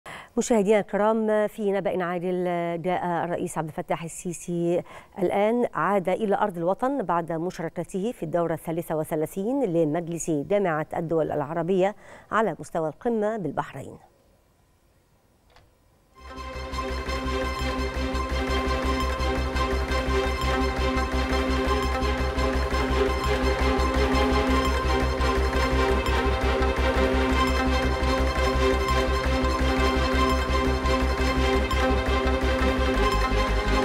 الرئيس السيسي يعود إلى أرض الوطن بعد مشاركته في الدورة الثالثة والثلاثين لمجلس جامعة الدول العربية
نشرة التاسعة | الرئيس السيسي يعود إلى أرض الوطن بعد مشاركته في الدورة الثالثة والثلاثين لمجلس جامعة الدول العربية على مستوى القمة بالبحرين